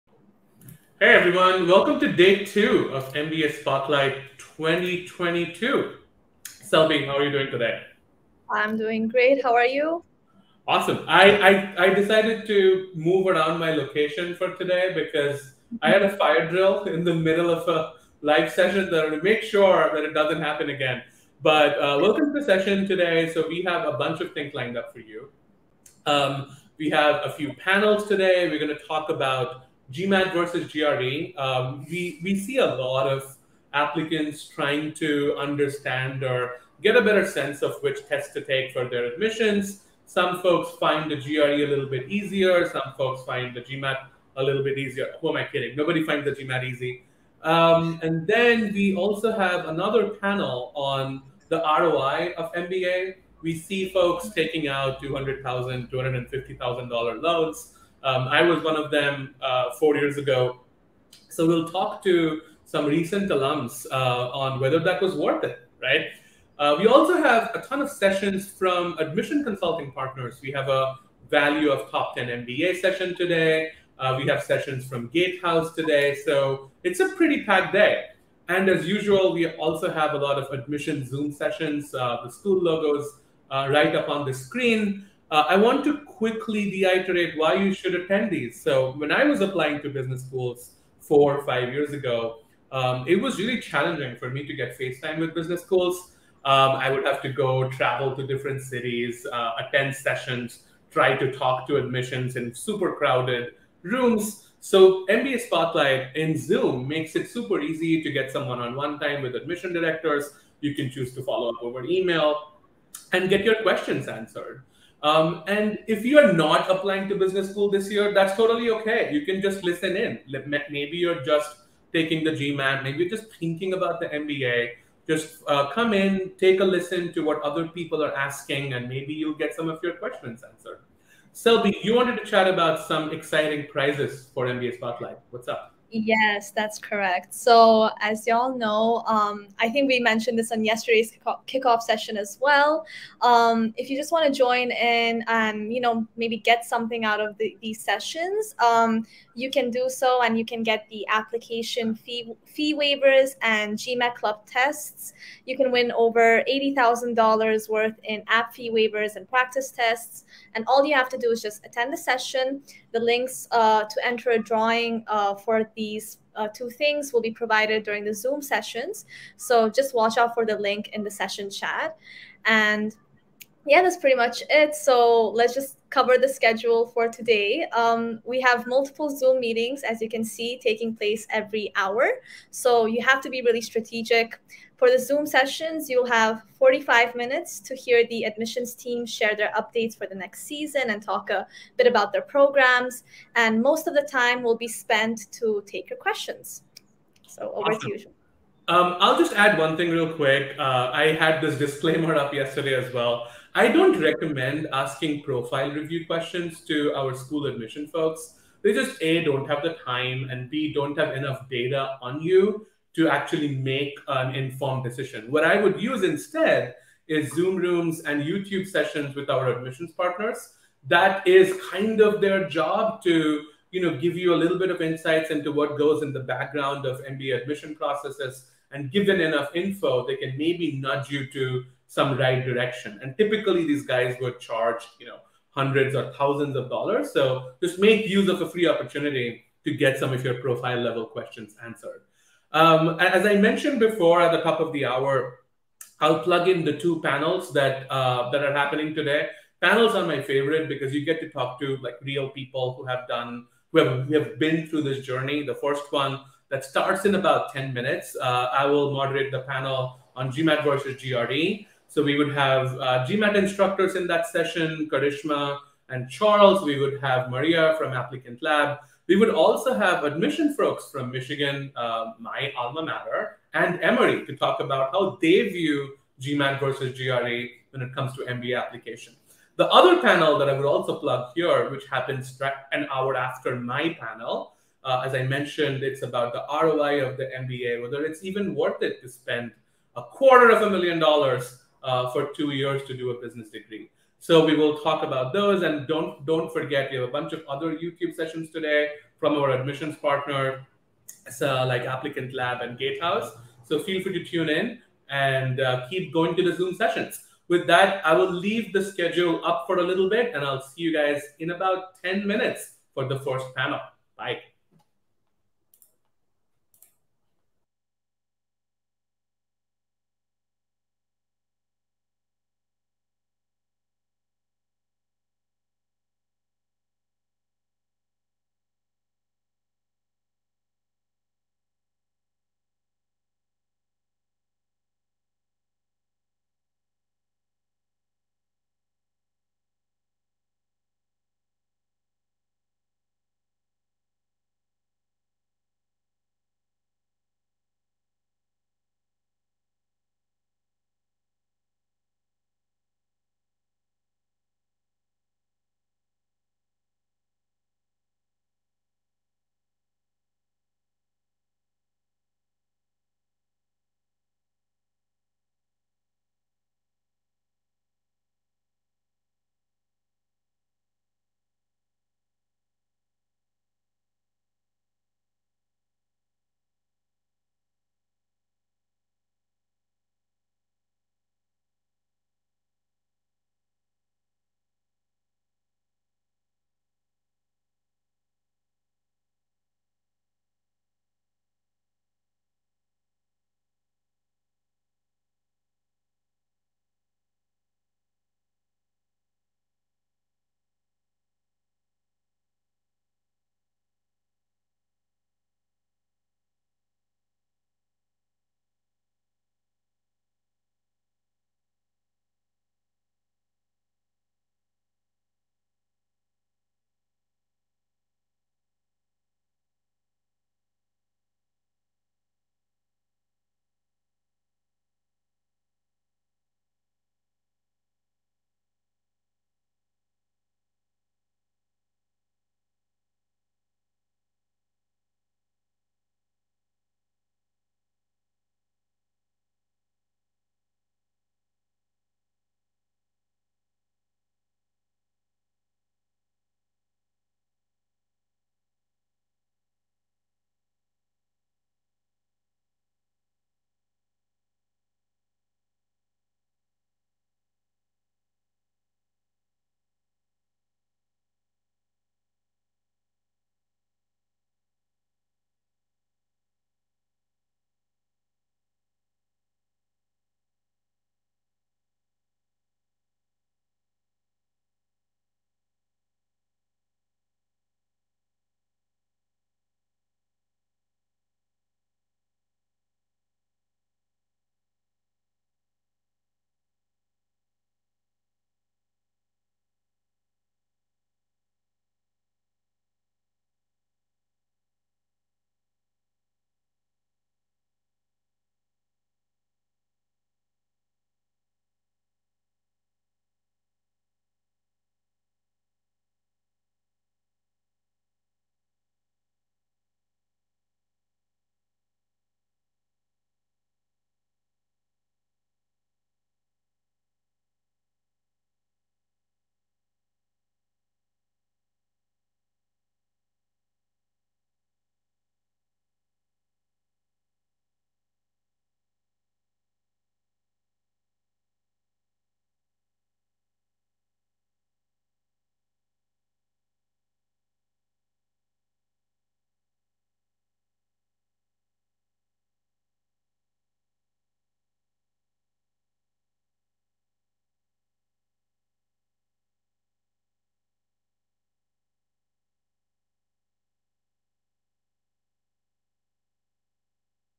MBA Spotlight Day 2- Overview of AdCom sessions, Panel discussions happening on Day 2
We have opened a fresh limited number of slots for day 2 of MBA Spotlight. REGISTER for MBA Spotlight to Secure your Spot

Souvik, Charles, and Selbi provide a brief snapshot of activities happening on the day 1 of MBA Spotlight and prizes.